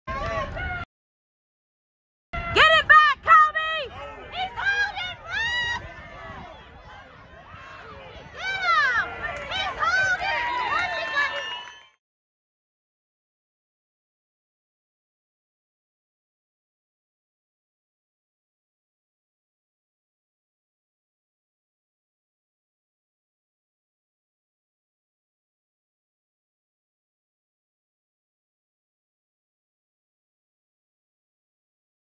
ref rips off player's helmet during Texas high school football game
During the Texas high school football game between Whitney and Dallas Madison, a Whitney player got tangled up with a ref and the ref intentionally yanked his helmet off. And then somehow, the Whitney player was penalized and ejected from the game...